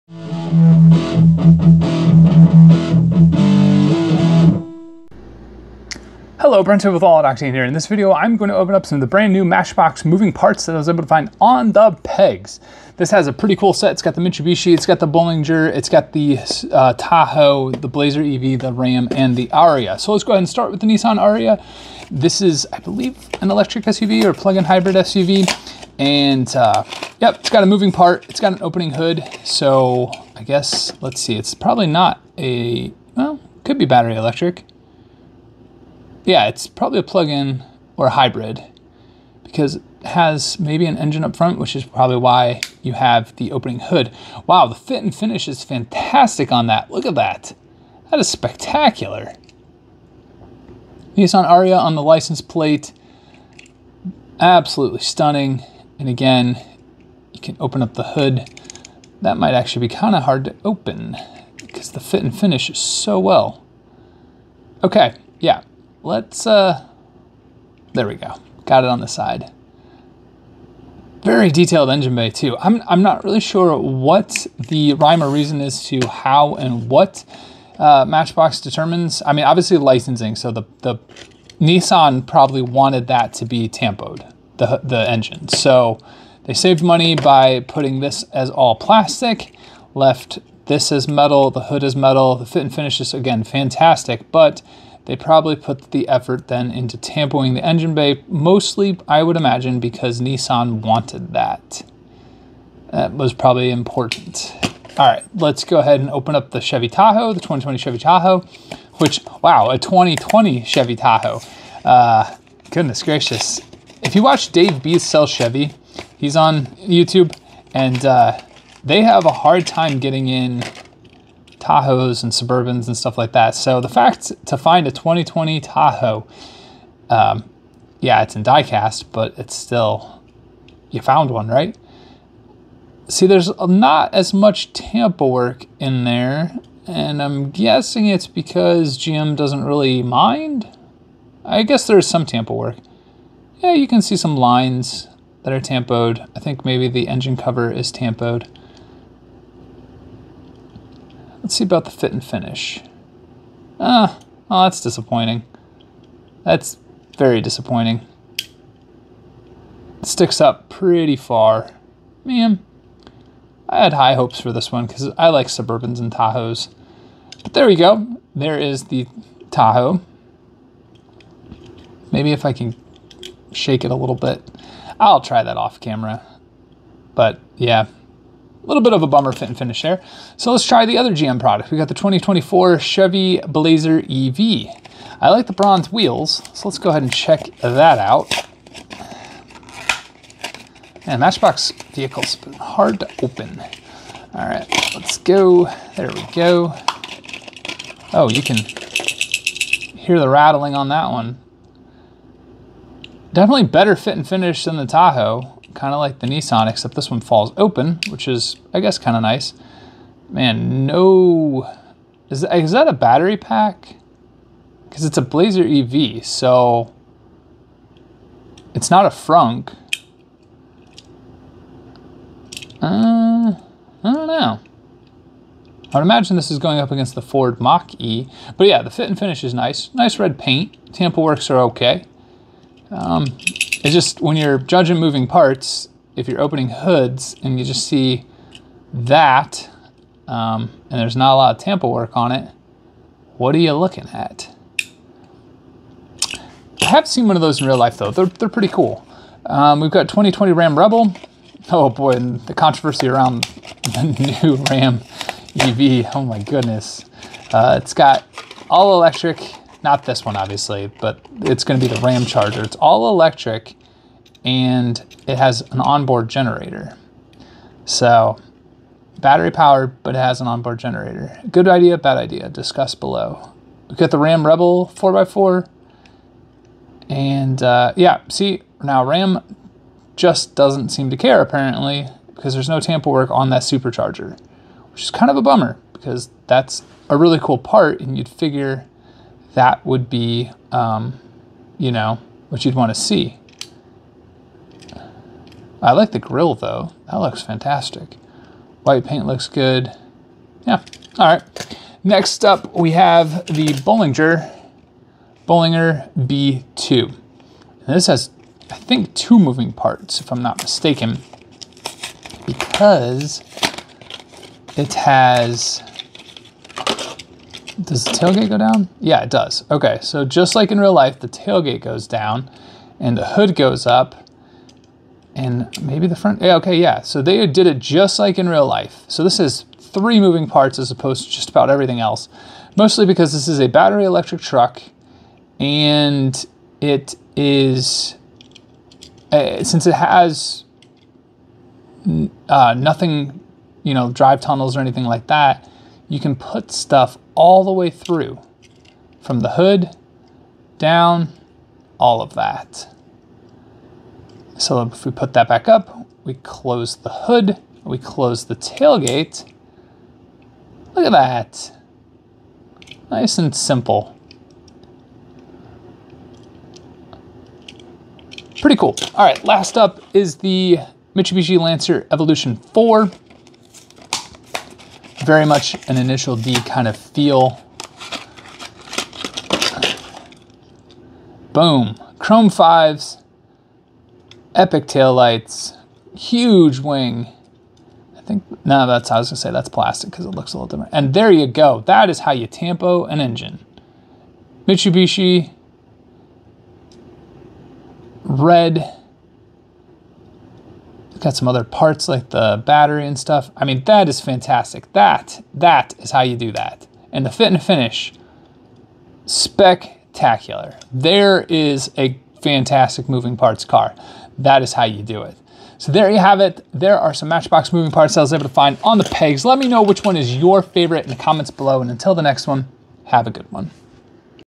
Opening Matchbox Moving Parts Subaru Bolinger Ram Rebel
In this video, I open up some 2023 Matchbox moving parts that I was able to find on the pegs! This set has the Ram Rebel, Subaru WRX, Chevy Blazer EV, Chevy Tahoe, and Bolinger pickup.